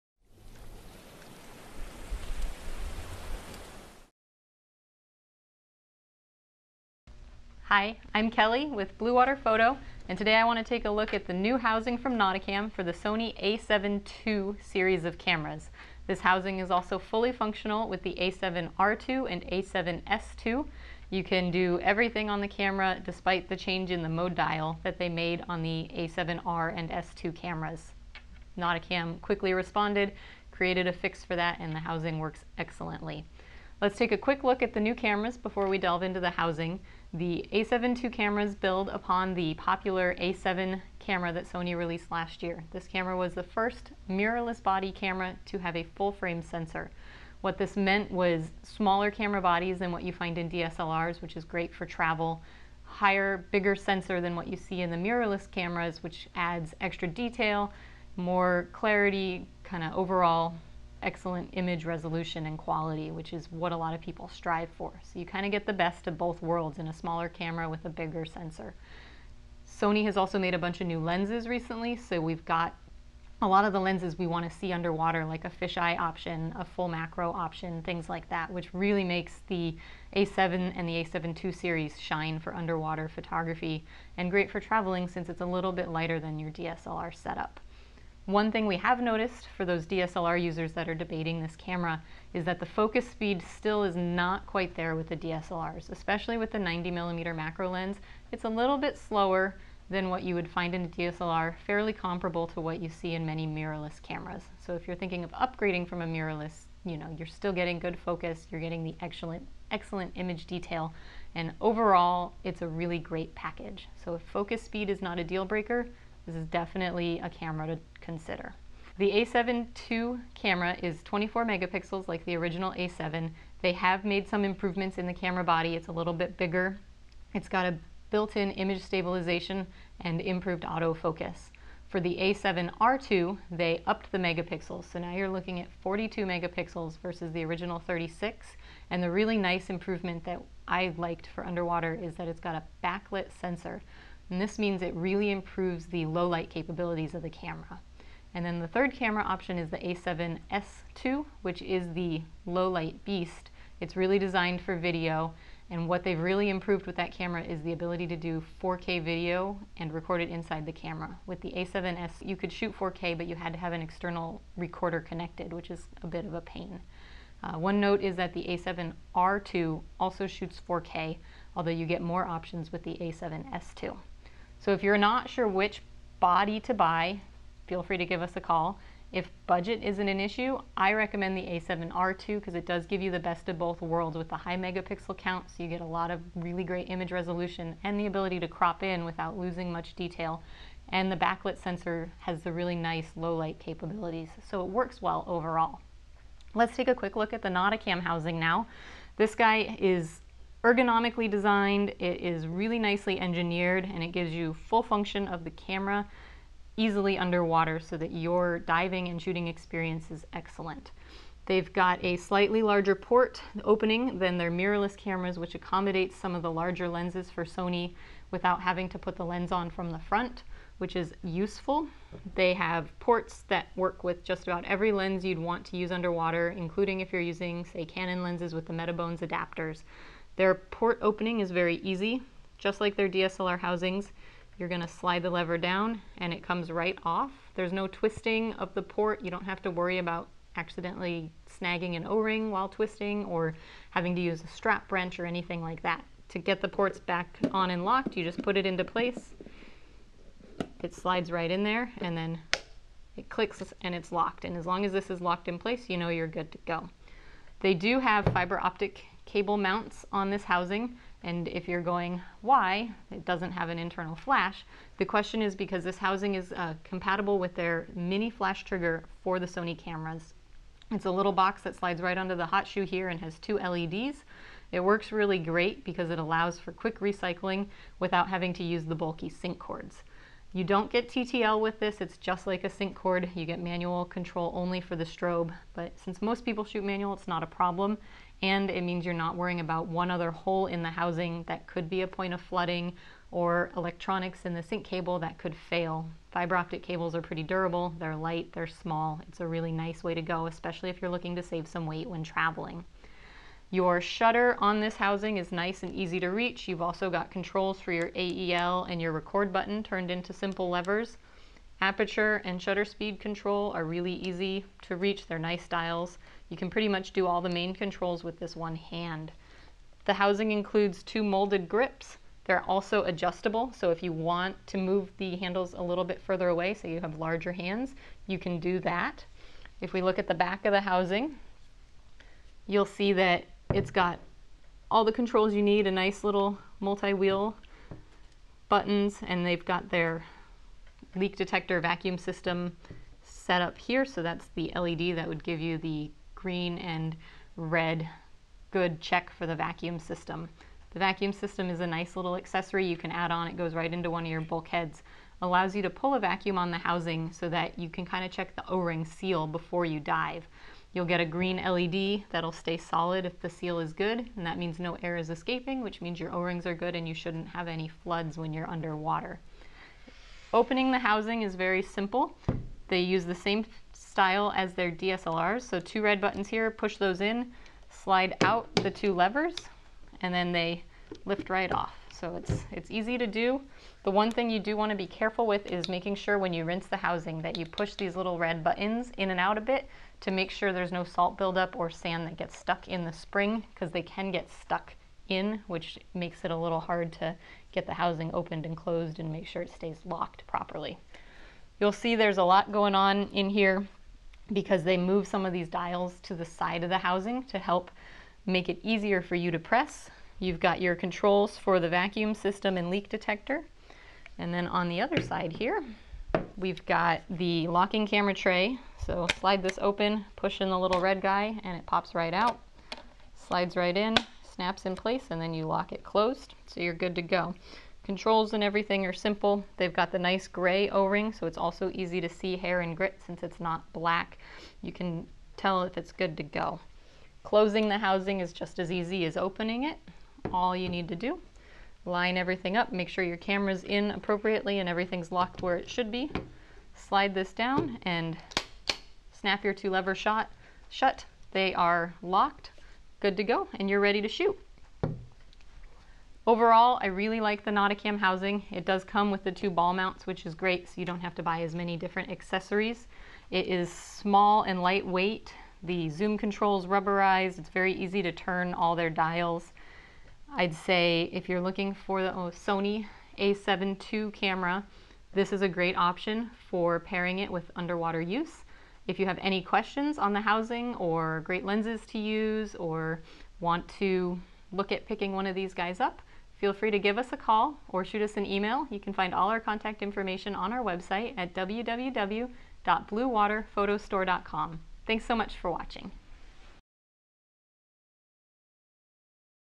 Nauticam Sony A7 II / A7R II Housing Review
Full review of the Nauticam A7II Housing and Sony camera.